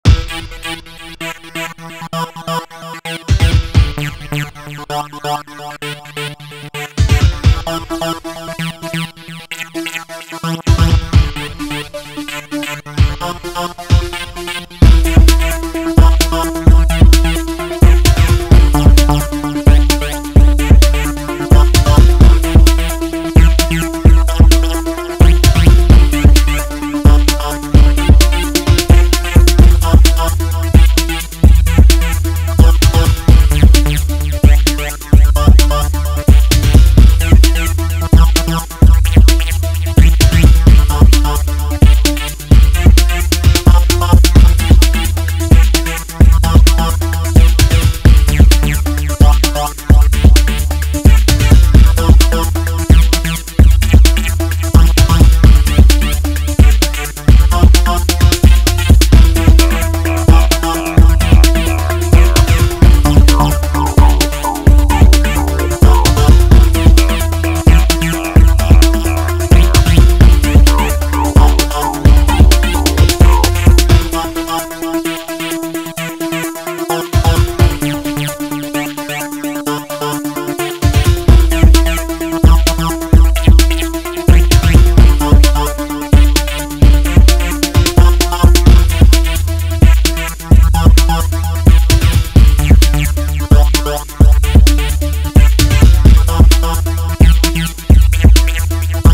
TIMELAPSE - Odlew dachu Audi R8
Wykonanie odlewu dachu samochodu Audi R8